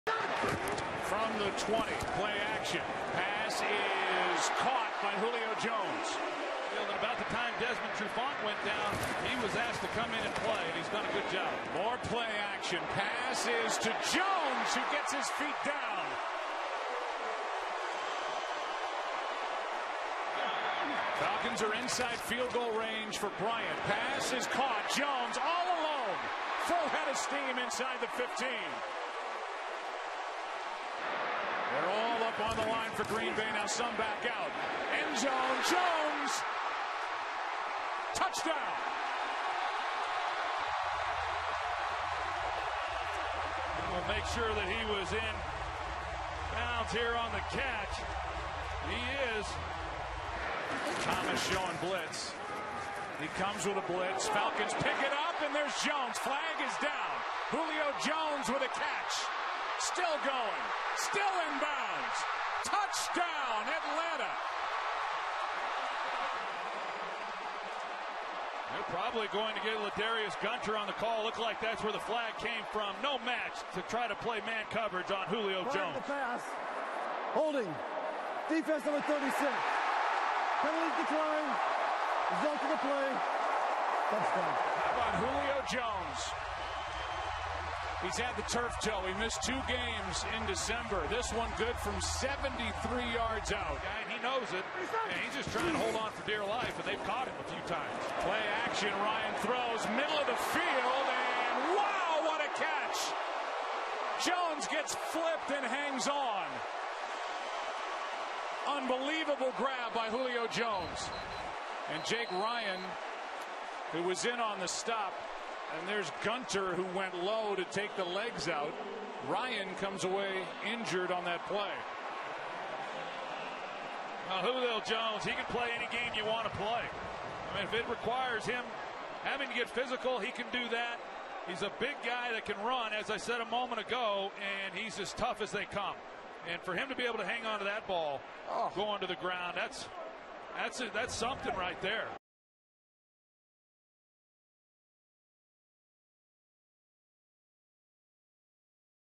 Julio Jones Goes Off for 180 Yards & 2 TDs! | Packers vs. Falcons | NFC Championship Highlights
Atlanta Falcons wide receiver Julio Jones reeled in 9 catches for 180 yards and 2 touchdowns in the Falcons win over the Green Bay Packers in the NFC Championship game.